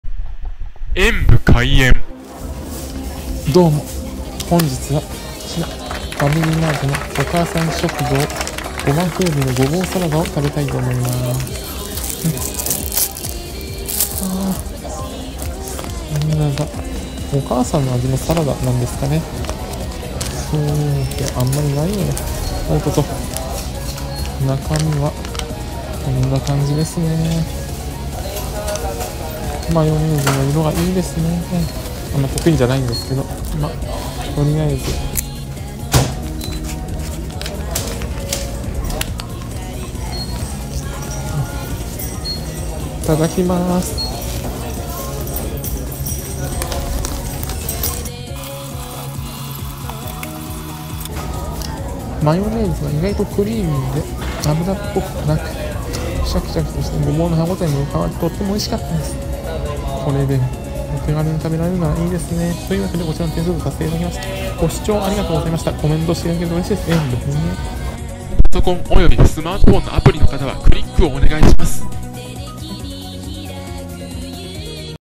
ファミリーマート お母さん食堂胡麻風味のごぼうサラダを食べてみた。ごマヨネーズが本当に香ばしい～
ファミリーマート　お母さん食堂胡麻風味のごぼうサラダを食べてみました。ゴママヨは意外と円やかで牛蒡と合います～
贈り物は此方まで↓
〒153-0042